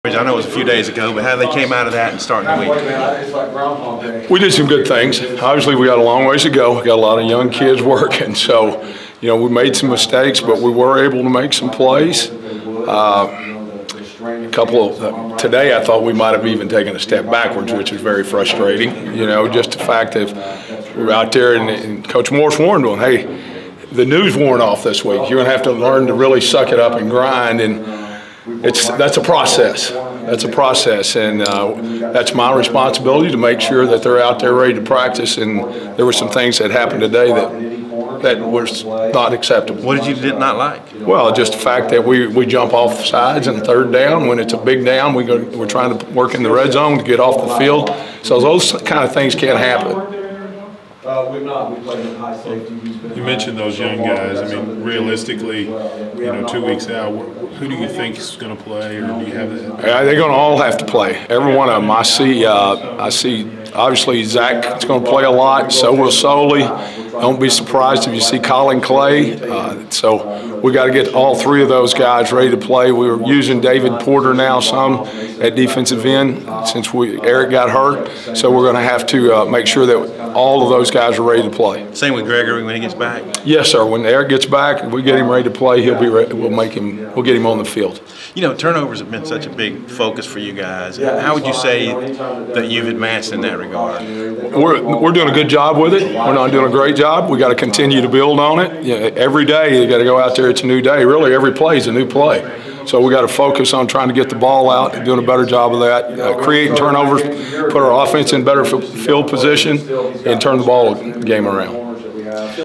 PRACTICE: Caldwell not pleased with group's practice Tuesday
Arkansas defensive ends coach Steve Caldwell talked after Tuesday's practice and he was not happy with how the workout went.